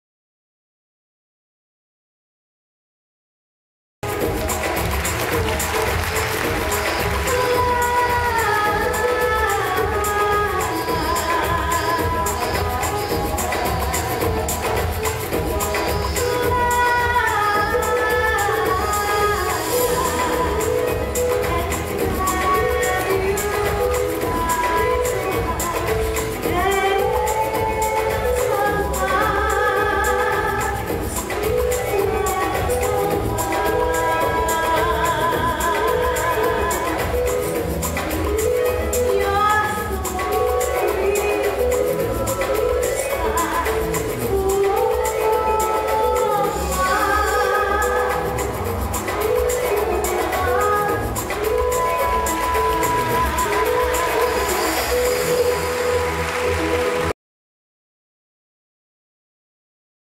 H.V. Quenzer Agency: Acrobat/Aerialist (Part 2)
Demo reel clip showcasing acrobat/aerialist (Part 2). Represented by H.V. Quenzer Agency.